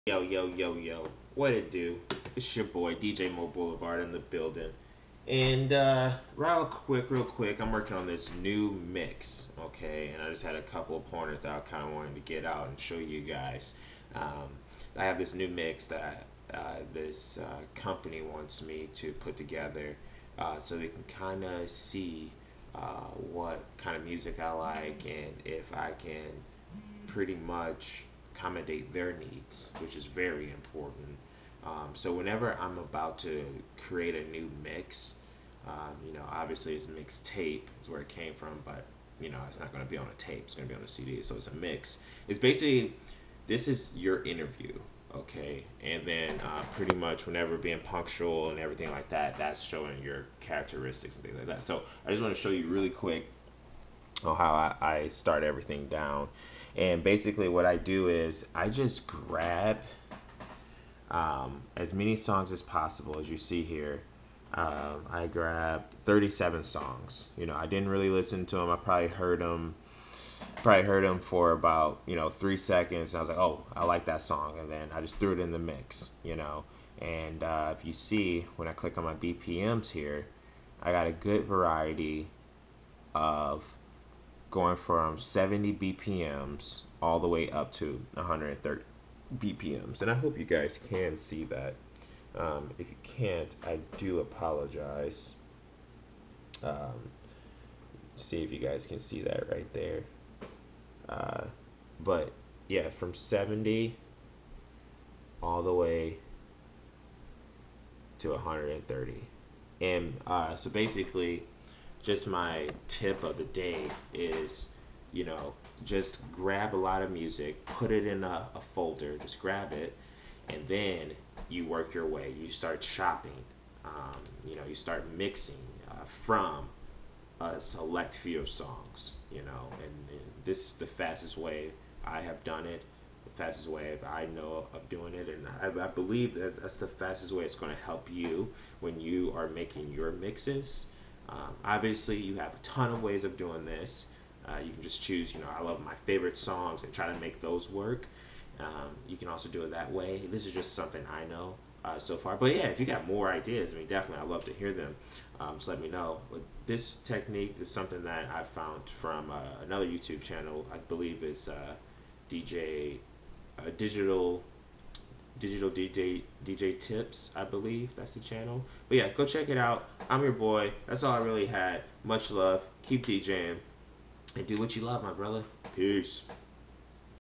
LifeToday: First step, when making a mix
So when you're making your mix. The first thing you want to do is find all the songs you want. Then from there see what songs work together. After that, be creative when taking one song out and bring the next one in (mixing).